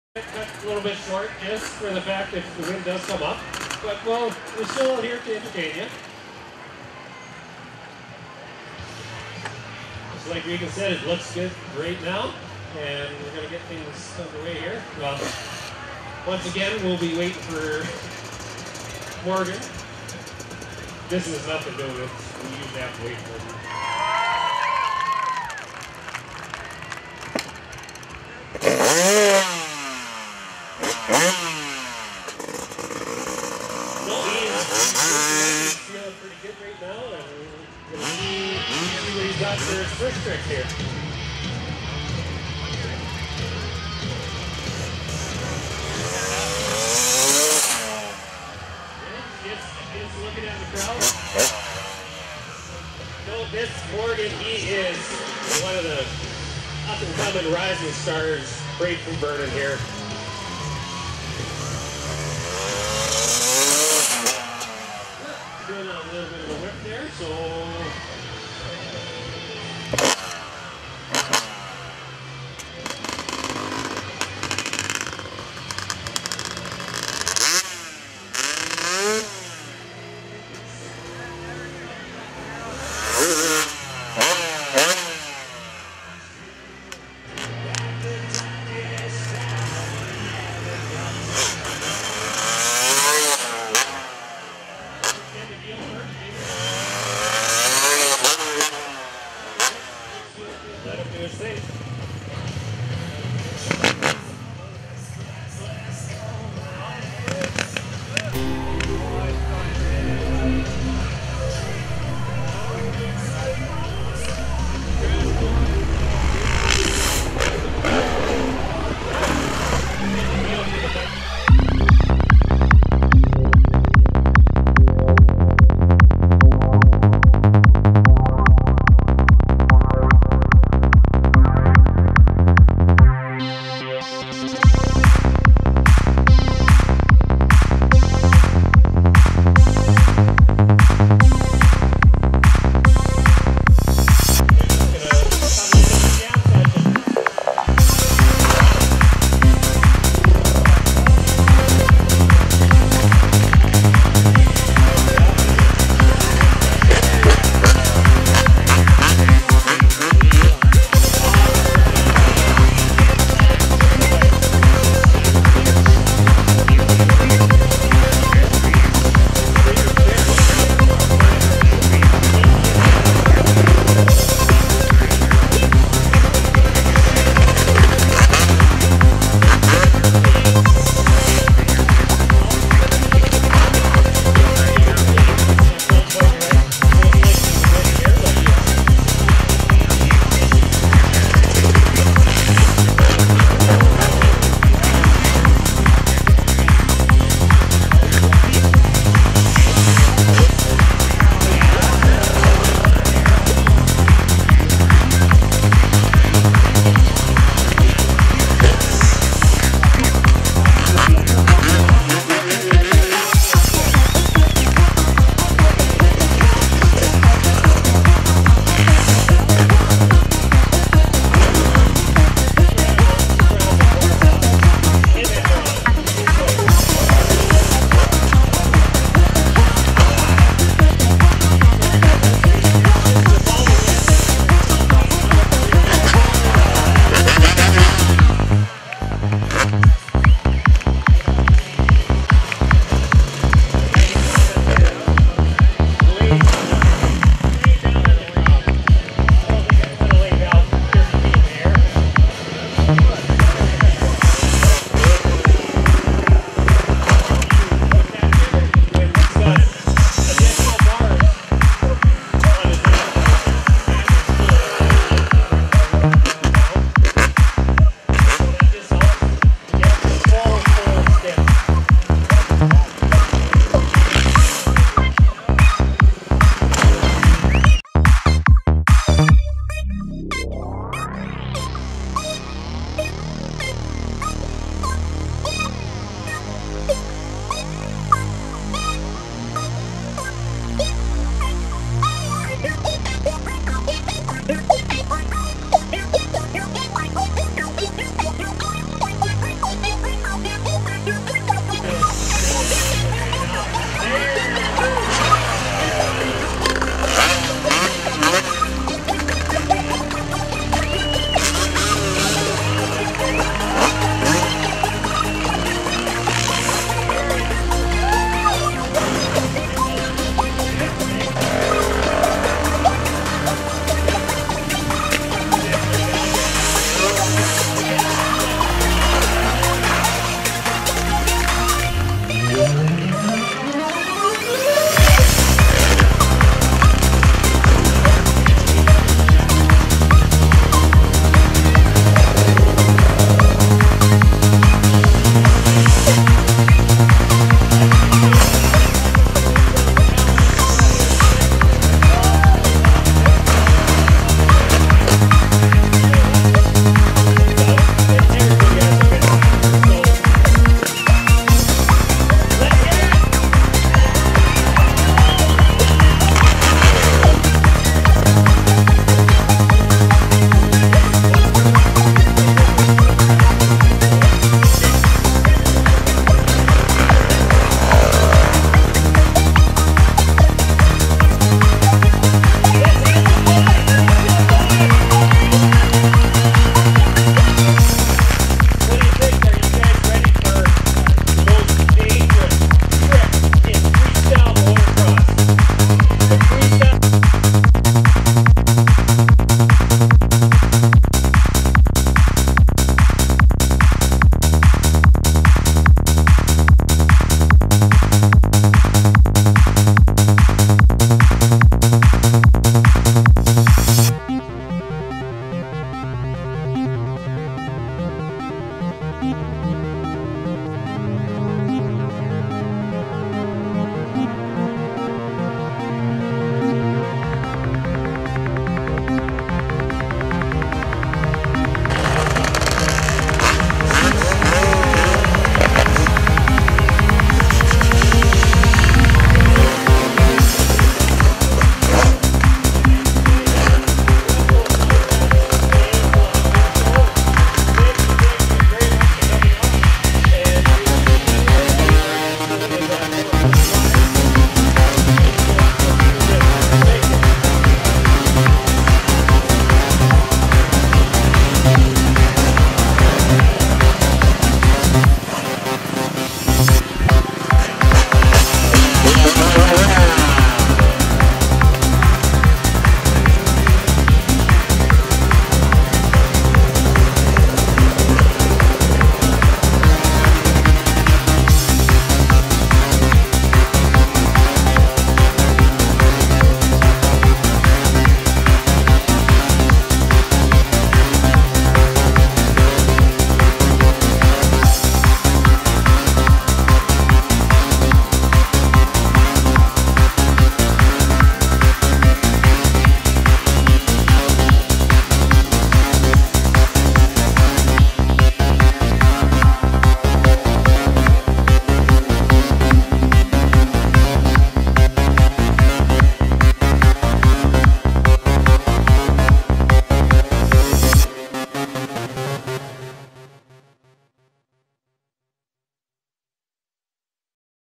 Jolly Jumpers 3 HD
stunt riders Jeff Fehr Reagan Sieg Morgan KaliszukJolly Jumpers 3 -  This is the 3rd year that we have filmed the Jolly Jumpers - freestyle motocross action at Lumby Day's, the video is very popular around the world with  nearly 1 million views for the original, so here  we have the latest Jolly Jumpers filmed June 11, 2012 in Lumby BC.